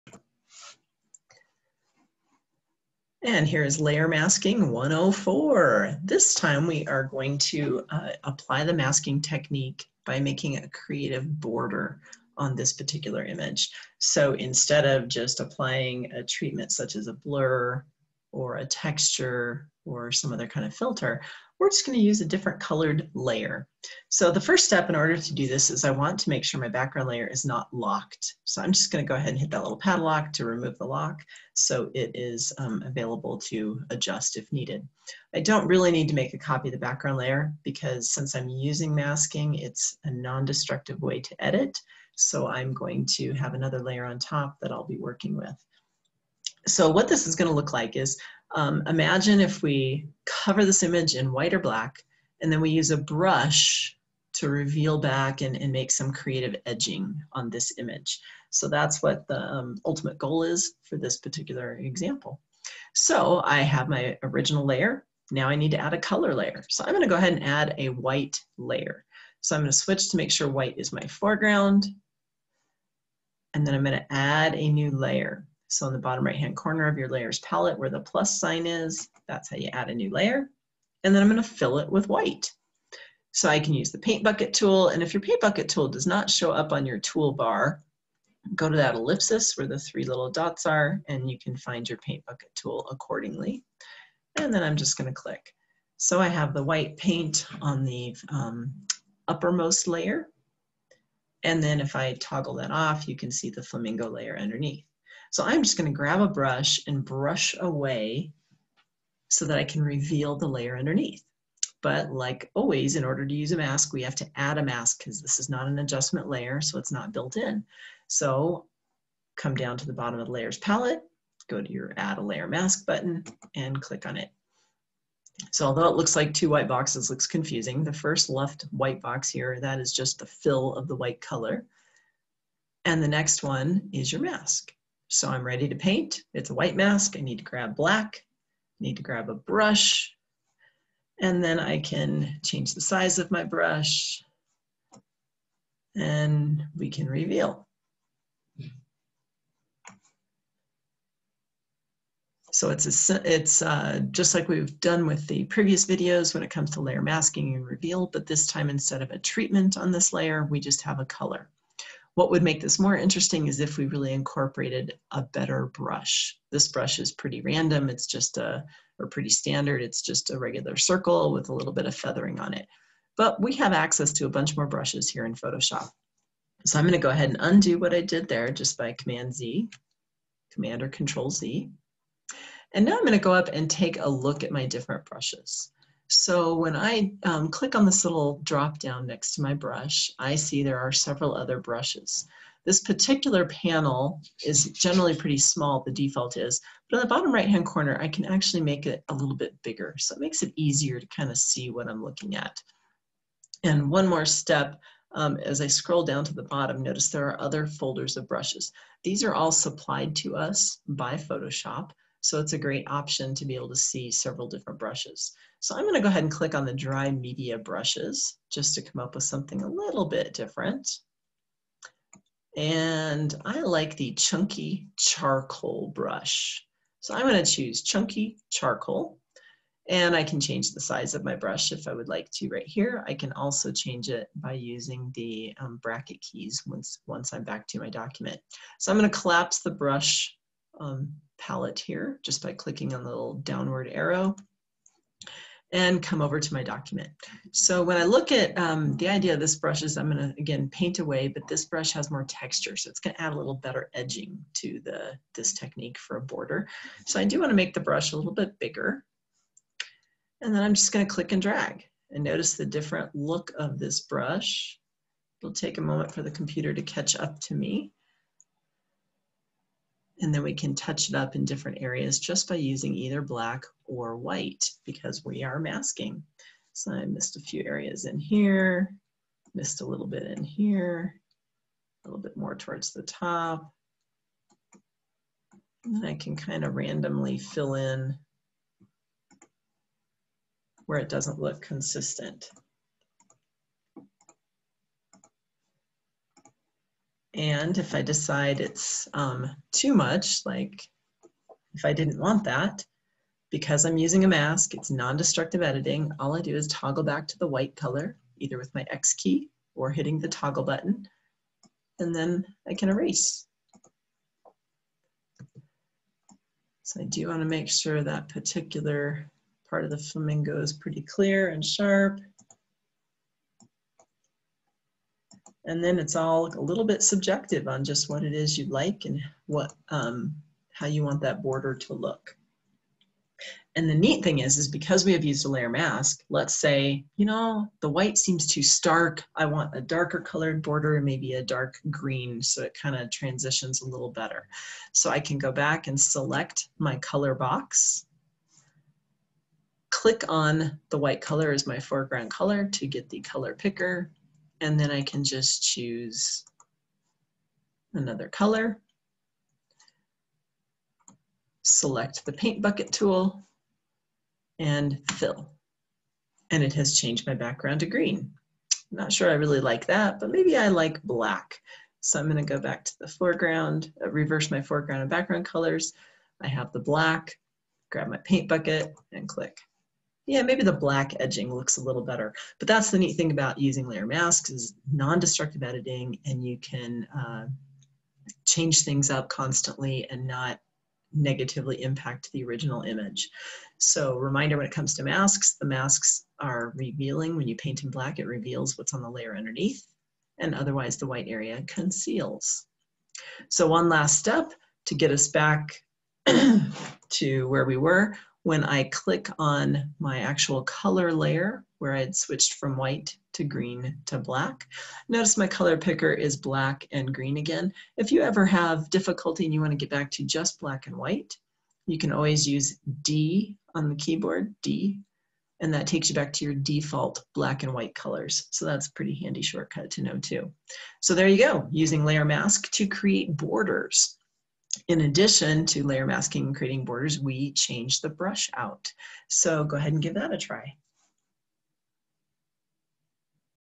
Photoshop Layer Masking 104
Amy Horn demonstrates layer masking using brushes to create unique borders in Photoshop.